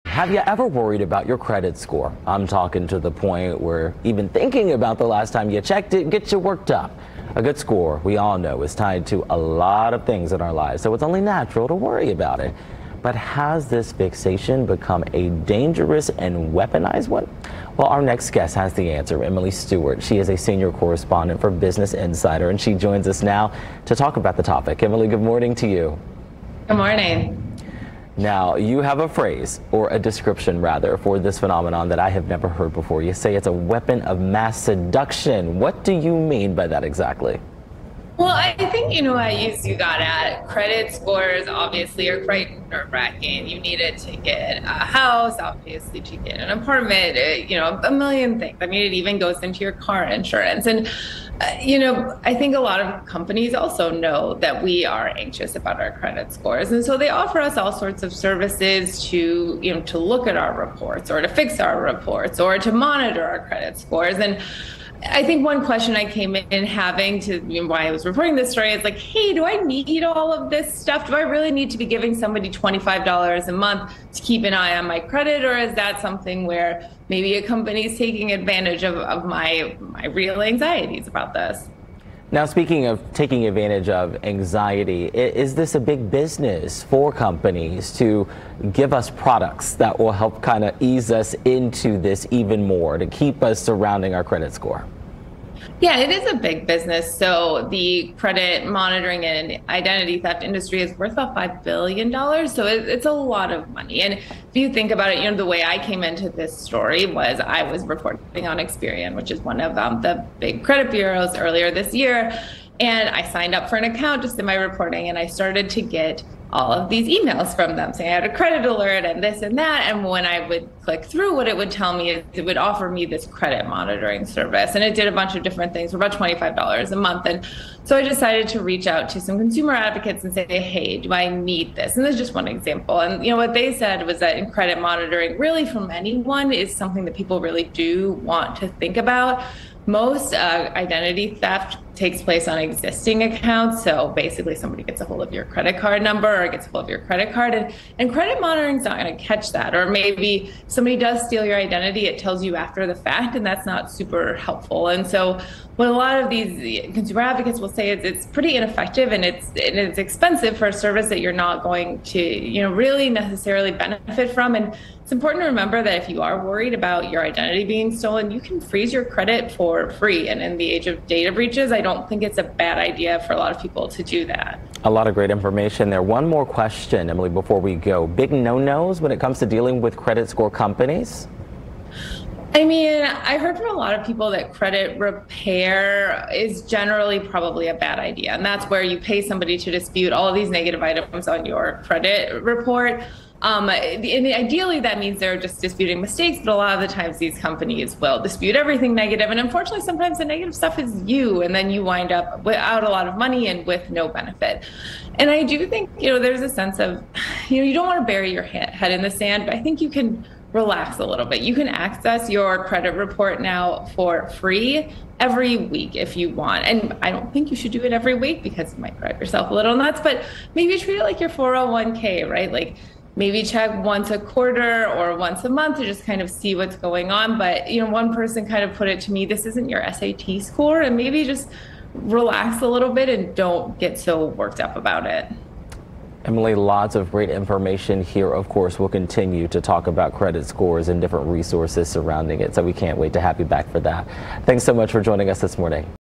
Why your credit score is crafted to cause you anxiety
Credit scores can be a source of anxiety for many Americans. But is this concern justified, or are consumers being manipulated into unnecessary expenses and anxiety?